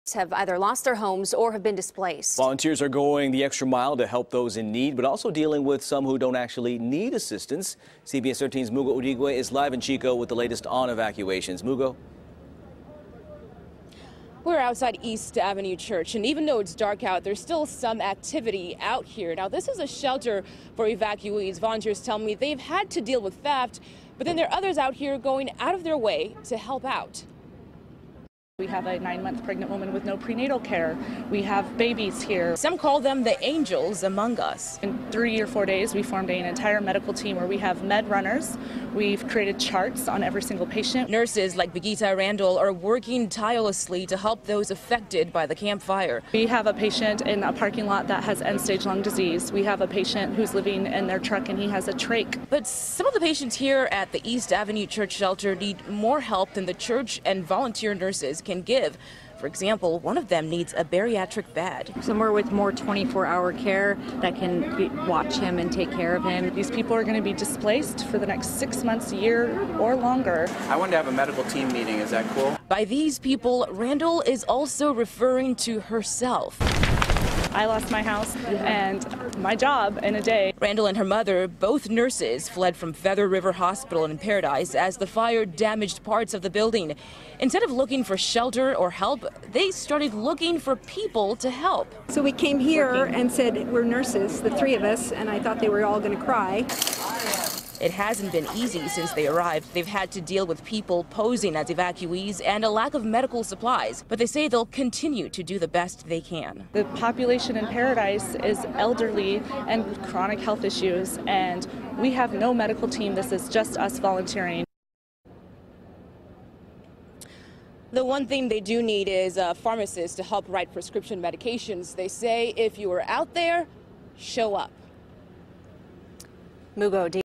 Volunteers Working To Care For Patients At Chico Shelter
Volunteers at East Avenue Church are going the extra mile to help those in need but also dealing with some who actually don’t need assistance.